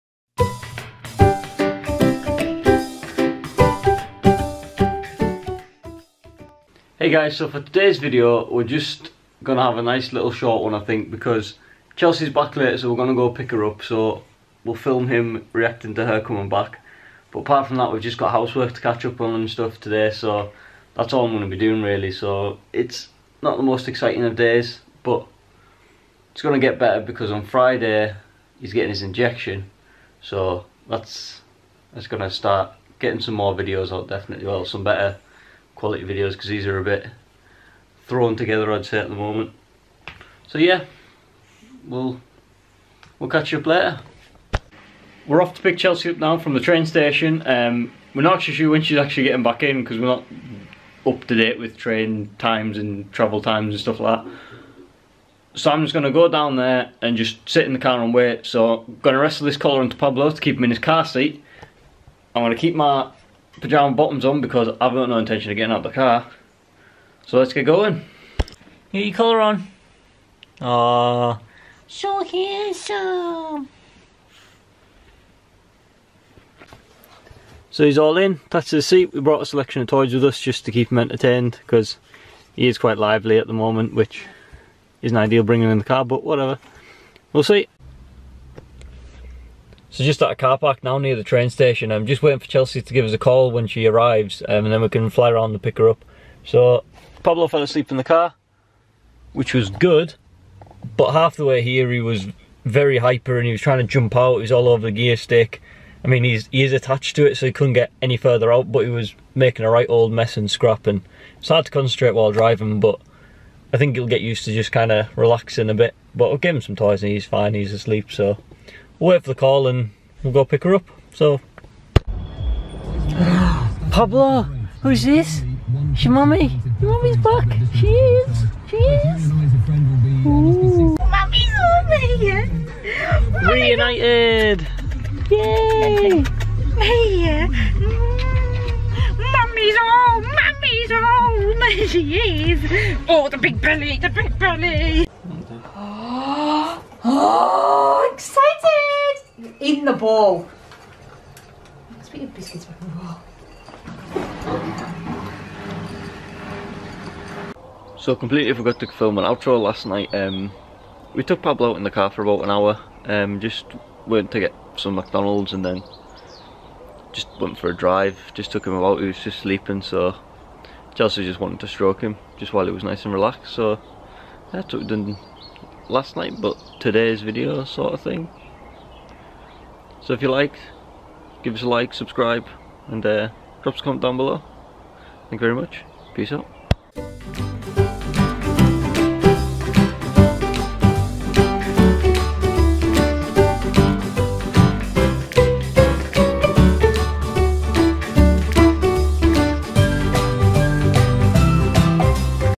Mammy Returns!
Chelsey is home from her trip away today! Pablo was dead exiting to see her, just a short video today!

Licence;